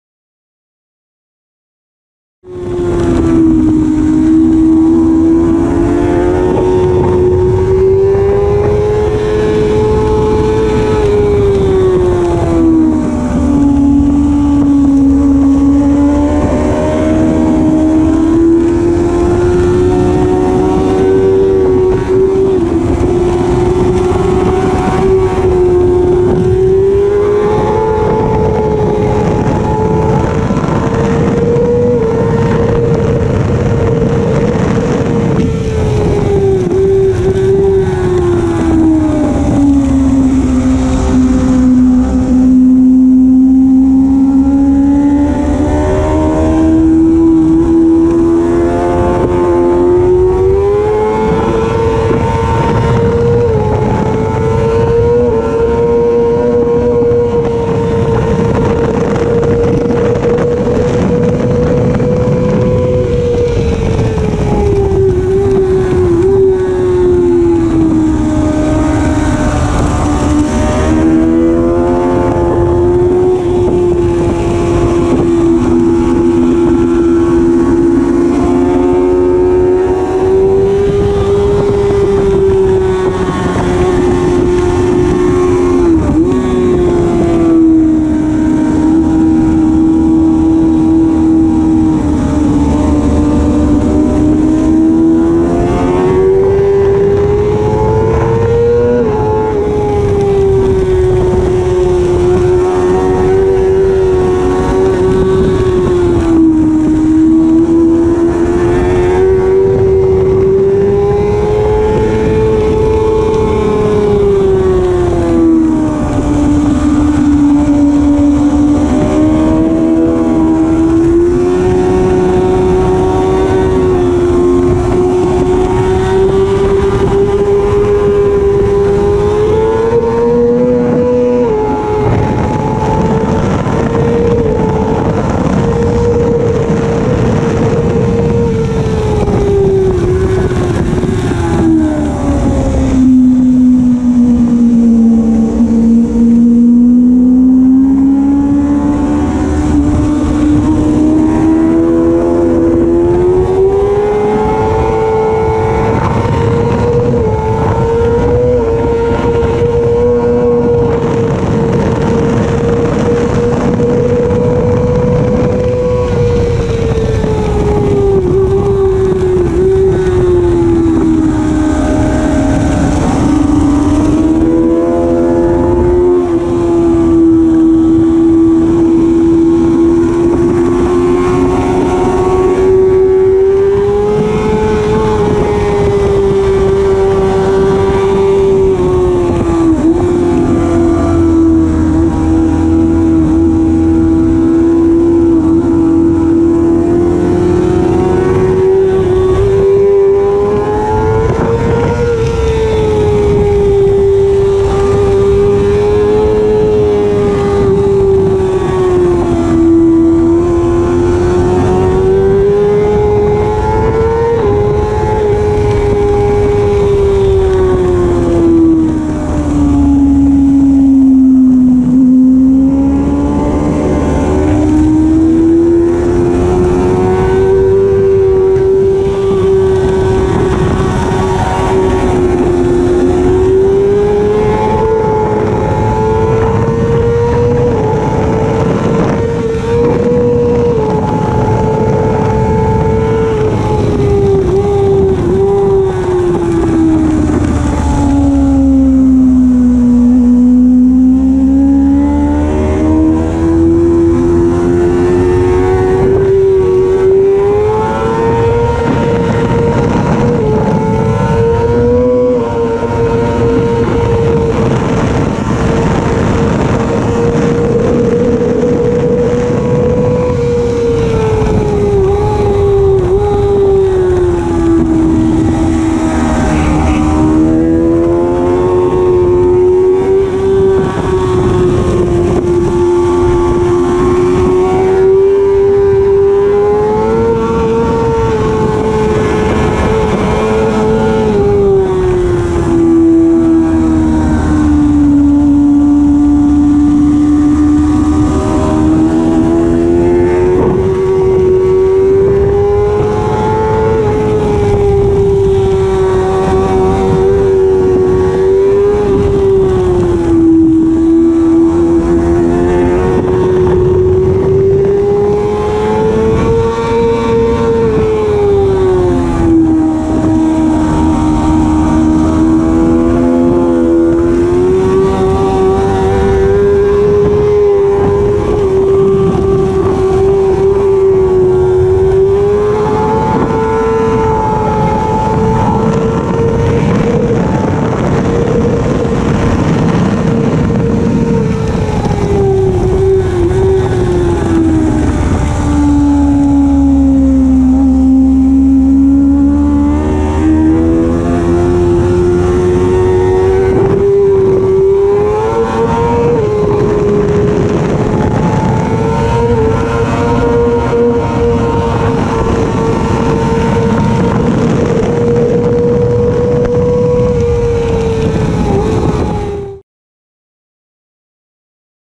Tutti in pista Rijeka 11.05.2019
sta volta senza gara ci accontentiamo di fare qualche giro con gli amici.
best 1.37